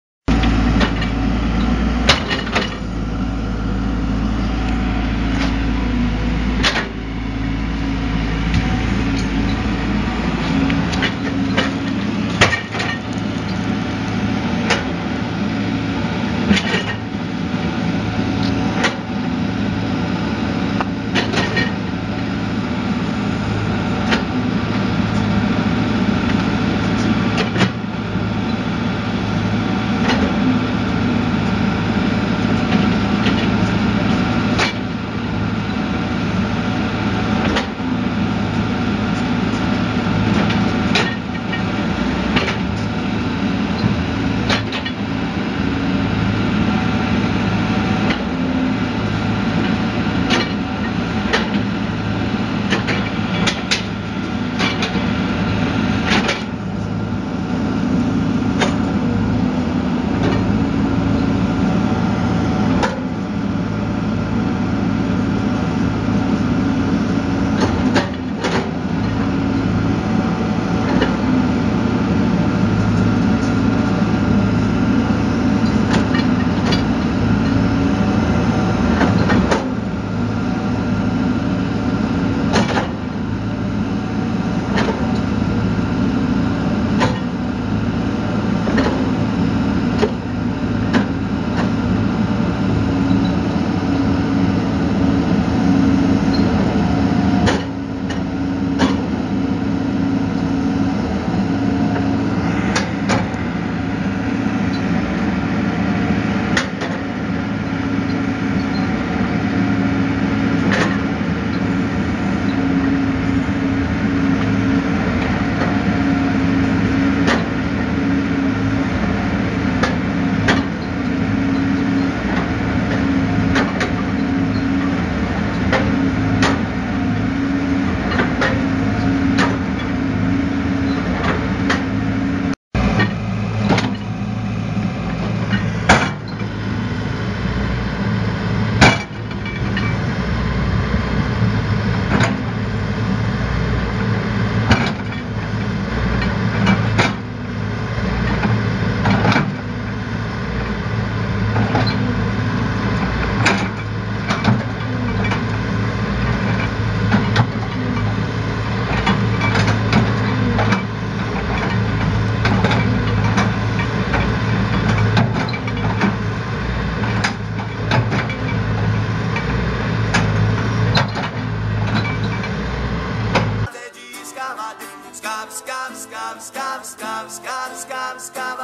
Profissionalismo com escavadeira hidráulica
Experiencia é o que conta em serviço dessa natureza, o operador da escavadeira é um profissional de qualidade.
Grupos no Facebook abaixo só os top das máquinas
OPERADORES DE EXCAVATOR e CRANE BRASIL: